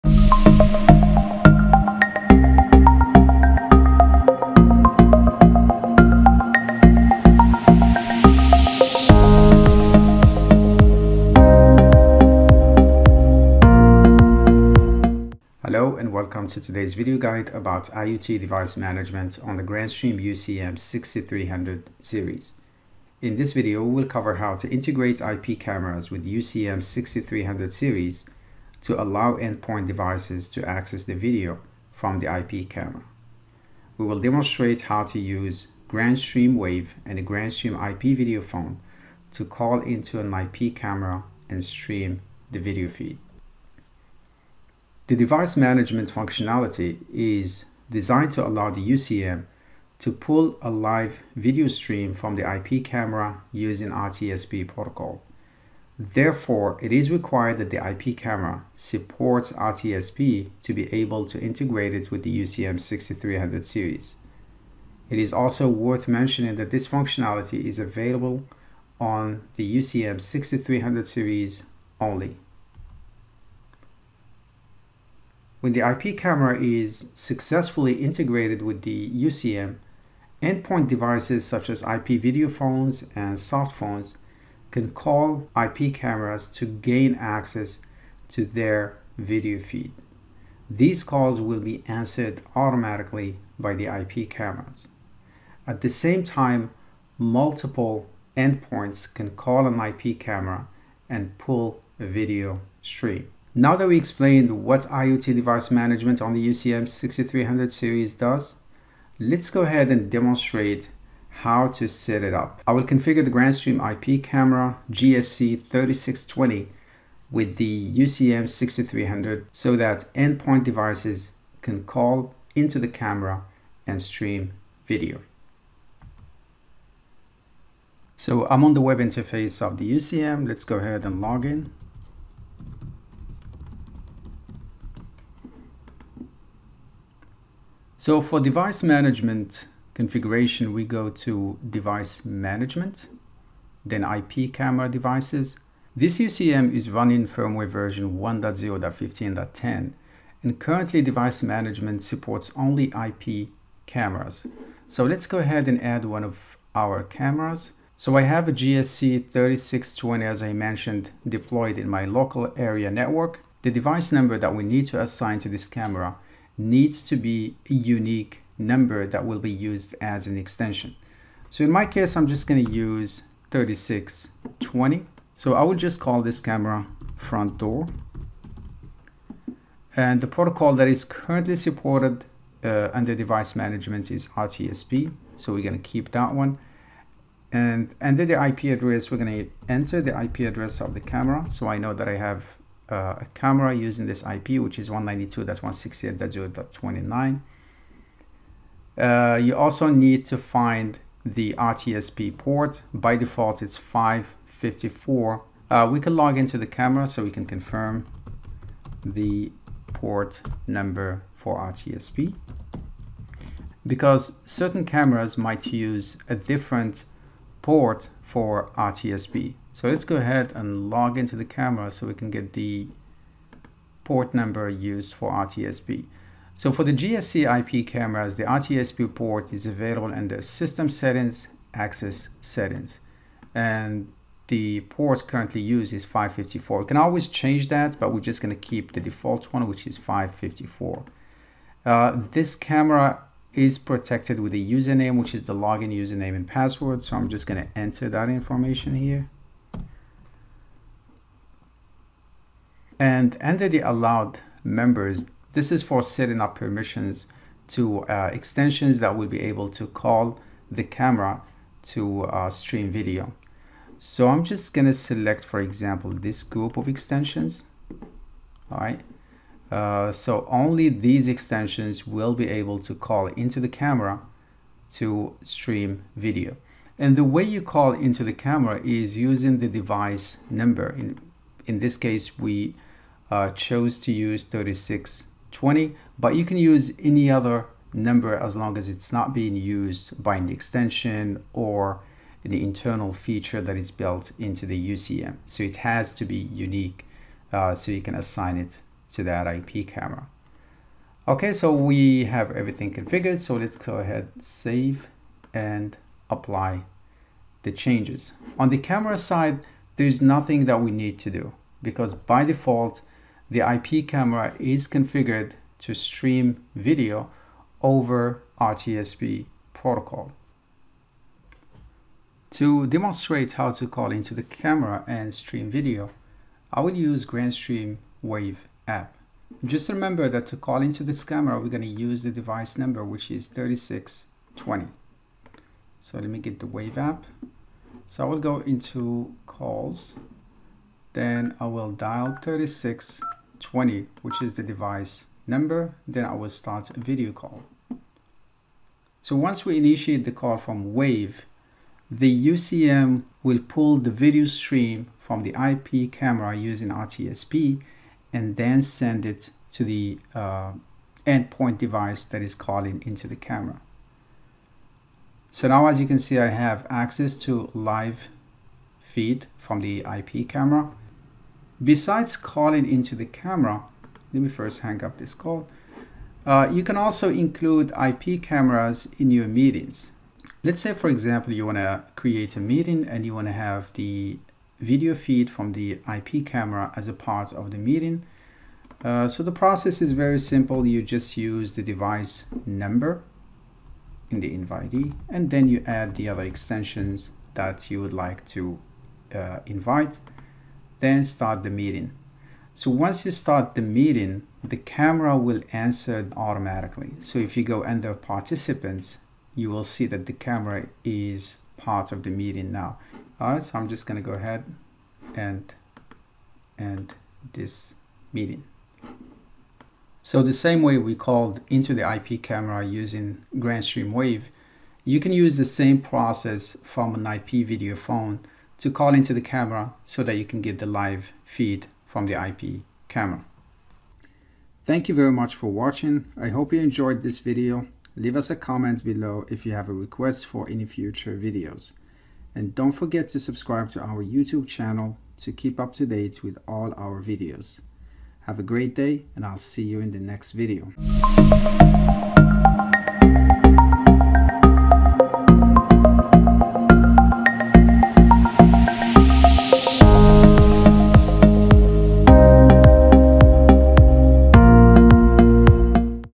Video Guides - IoT Device Management - UCM6300 Series
In this video, we cover IoT device management using Grandstream UCM6300 series and demonstrate how to integrate an IP camera with UCM6300 to stream video on endpoint devices. These are the points covered in this video:

- 00:30 IoT device management overview
- 01:52 Integrate IP camera with UCM6300: Configuration steps
- 05:20 Streaming IP camera video from endpoint devices